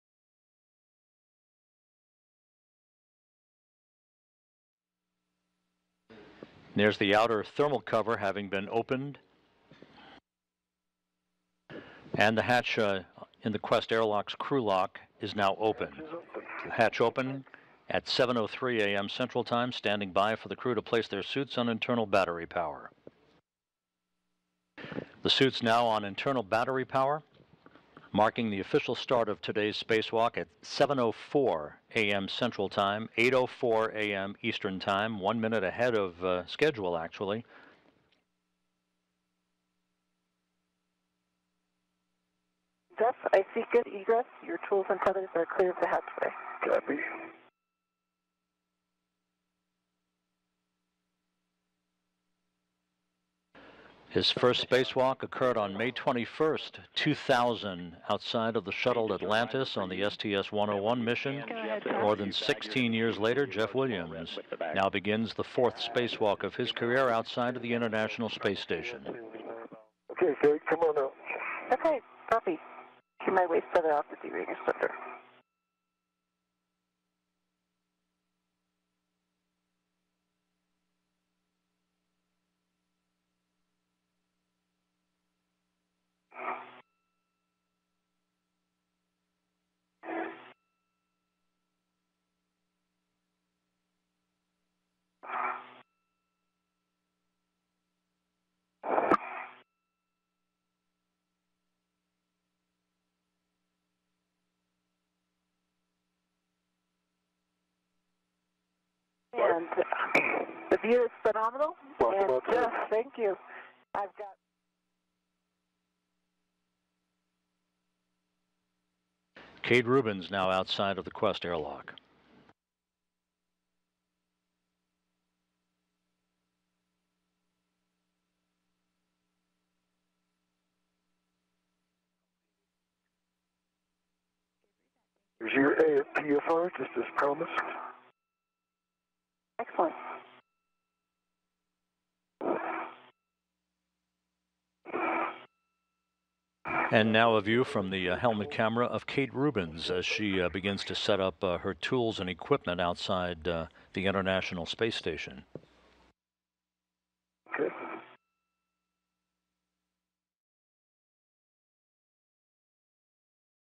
Kate Rubins starts first spacewalk
Astronaut Kathleen “Kate” Rubins started her first spacewalk (extravehicular activity - EVA) on 19 August 2016, at 12:04 UTC, along with NASA astronaut Jeffrey “Jeff” Williams to install the International Docking Adapter (IDA) to the International Space Station Harmony module.

Credit:
NASA